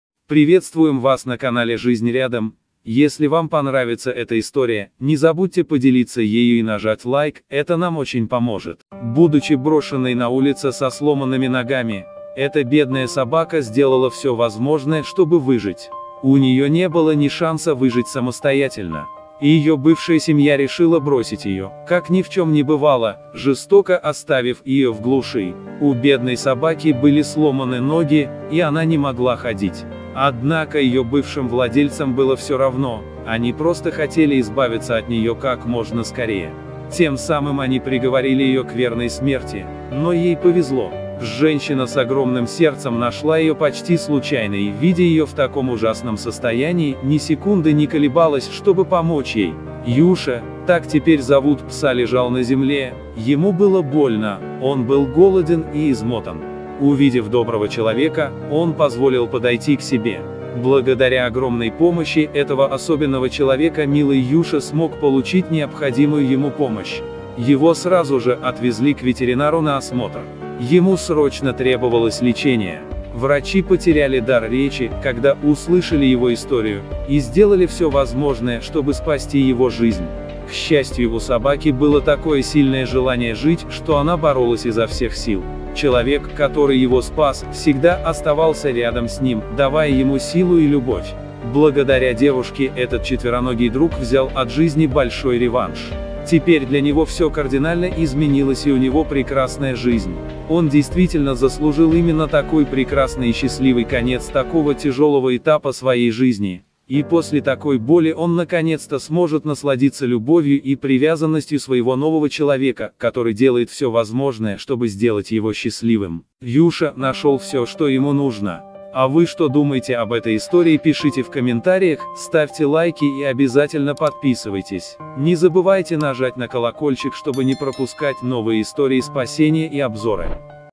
Хозяева решили от него избавиться и они выбрали самый страшный способ
Будучи брошенной на улице со сломанными ногами, эта бедная собака сделала все возможное, чтобы справиться. У нее не было ни надежды выжить самостоятельно, ни шанса сделать это.